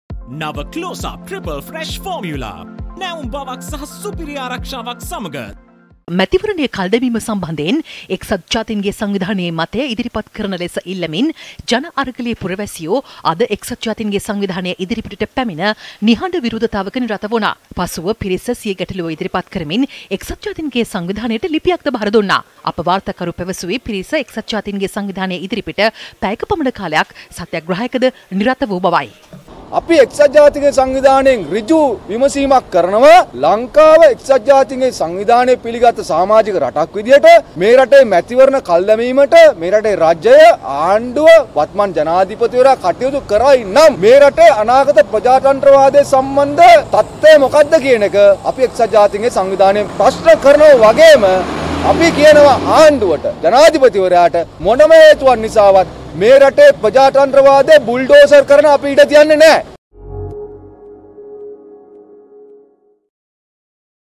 ඡන්දය ඉල්ලා එක්සත් ජාතීන්ගේ සංවිධානය ඉදිරිපිට විරෝධතාවක්
''ප්‍රජාතන්ත්‍රවාදය බුල් ඩෝසර් කරන්න, අපි ඉඩ තියන්නේ නැහැ'' - චානක බණ්ඩාර

මැතිවරණය කල් දැමීම සම්බන්ධයෙන් එක්සත් ජාතීන්ගේ සංවිධානයේ මතය ඉදිරිපත් කරන ලෙස ඉල්ලමින්, ජන අරගලයේ පුරවැසියෝ අද එක්සත් ජාතීන්ගේ සංවිධානය ඉදිරිපිටට පැමිණ නිහඬ විරෝධතාවයක නිරත වුණා.